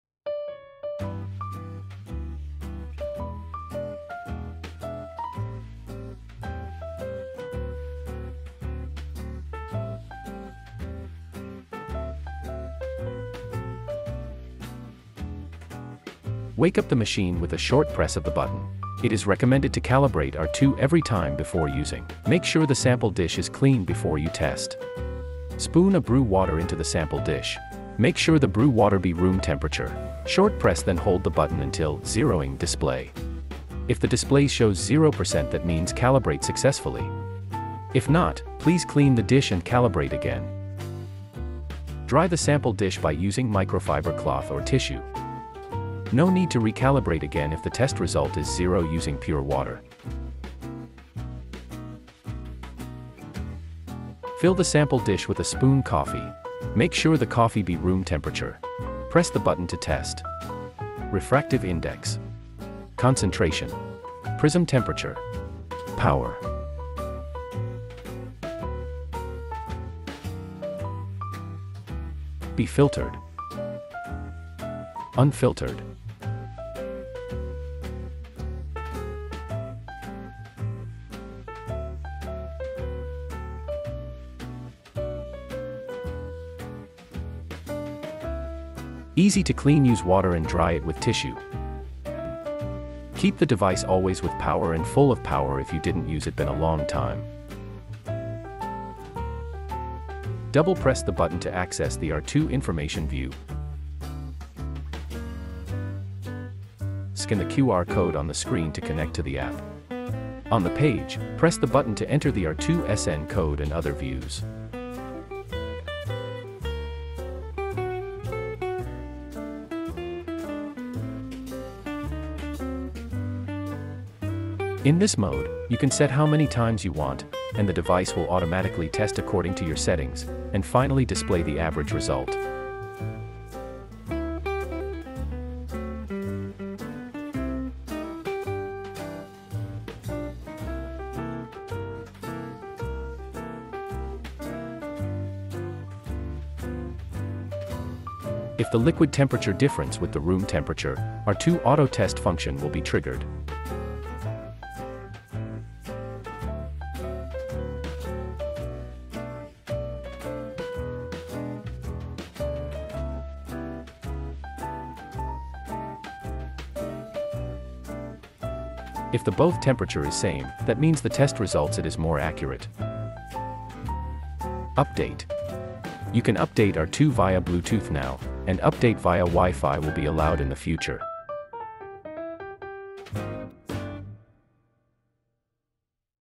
DiFluid R2 Extract Official Tutorial
00:00 Calibration
00:53 Testing
01:09 Espresso testing
01:29 Cleaning
01:40 Access system information
01:59 Average Test
02:23 Auto Test
02:39 Advance Temperature View
02:53 Update